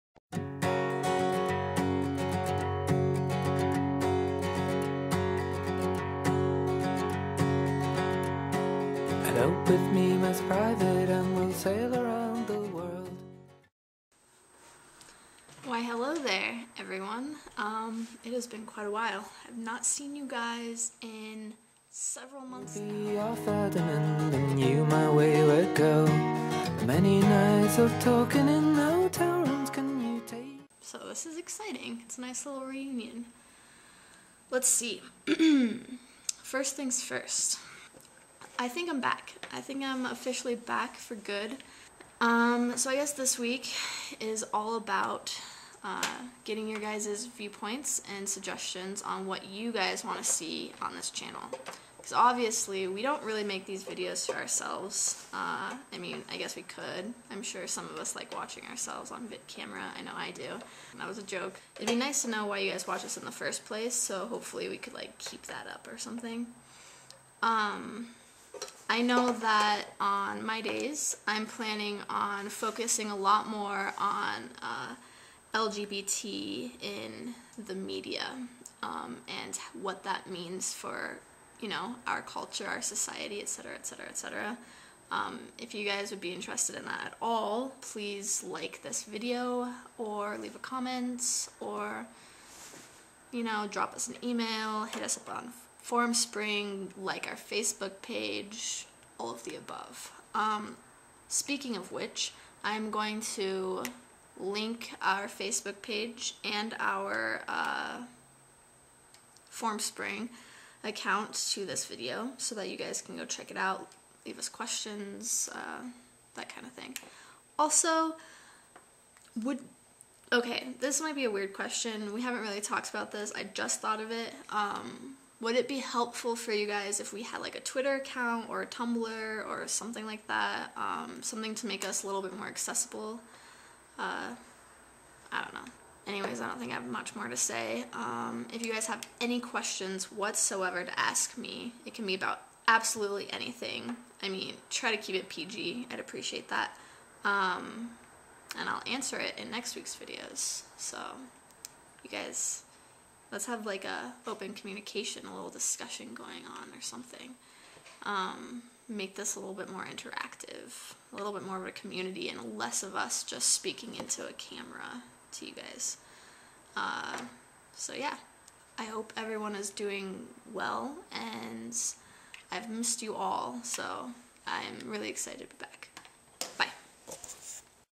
Ask Steph Questions! Absolutely ANYTHING.
So, I'm officially back for good now. I'll be taking over Bennett's spot (he just got a little too busy, and we'll all miss him!) so yay! I missed you guys! Anyways, ask me questions. Lots and lots of them! and here are the links for you guys... and let us know if you want us anywhere else (ie. tumblr, twitter, etc)